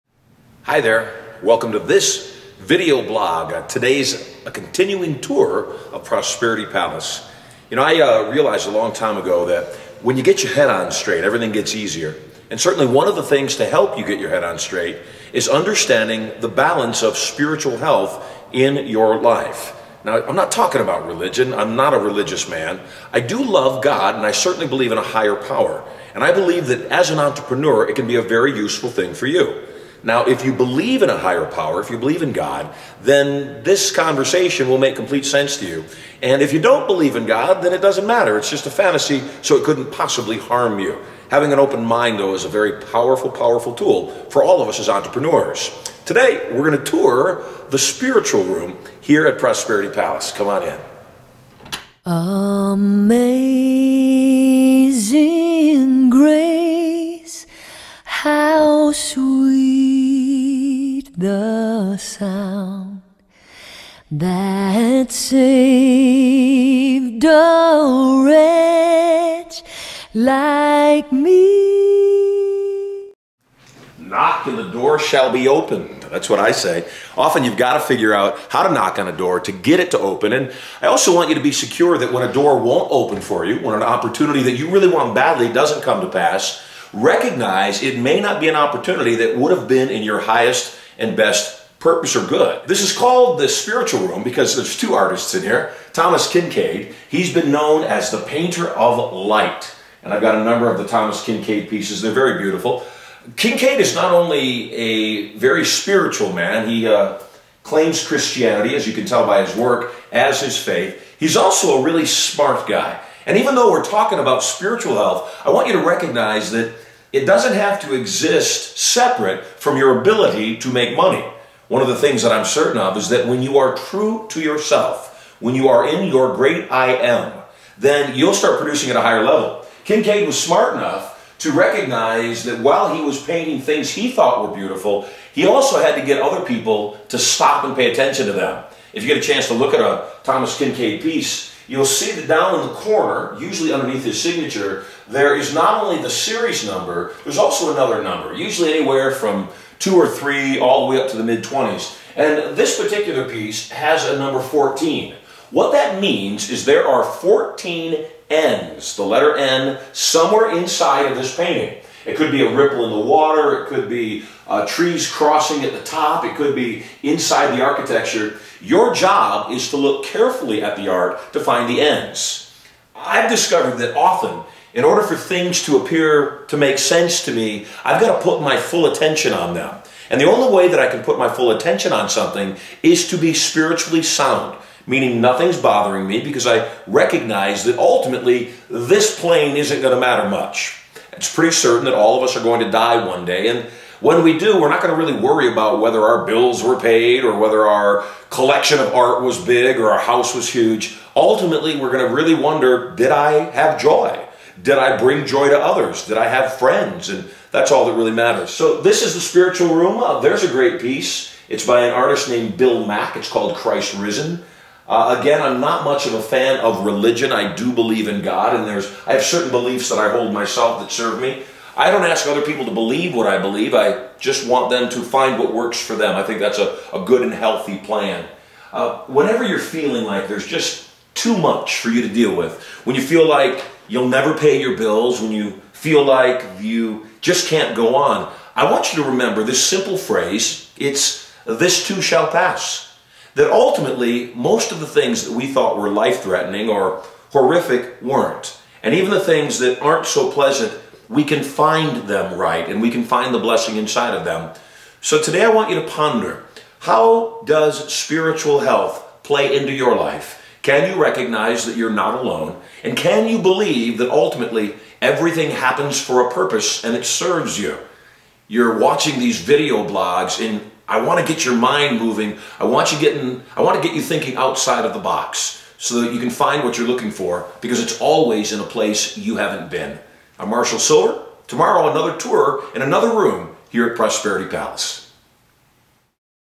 Lesson from *Marshall Sylver* - *Spiritual Health*
(Marshall Sylver) takes you on a tour of another room at (Prosperity) Palace - The (Spiritual) Room, while giving you a lesson on how (spiritual health) plays a role in your life & in your success.